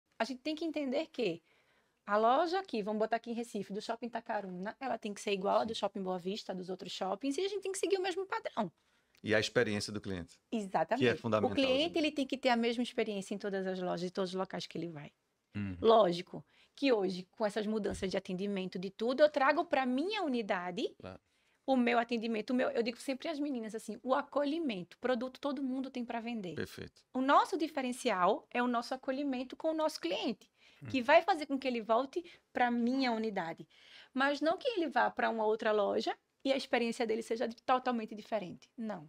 O cliente tem que ter a mesma experiência em todas as lojas 💡
Cristiana Vasco fala sobre a experiência do cliente e a importância desse trabalho para se destacar da concorrência. Ela explica que o diferencial da empresa dela é o acolhimento, pois produtos muitos vendem, mas eles oferecem um produto de qualidade e um acolhimento especial.

Caso queira atendimento personalizado, conte conosco! Basta preencher seus dados no link a seguir e entraremos em contato com você!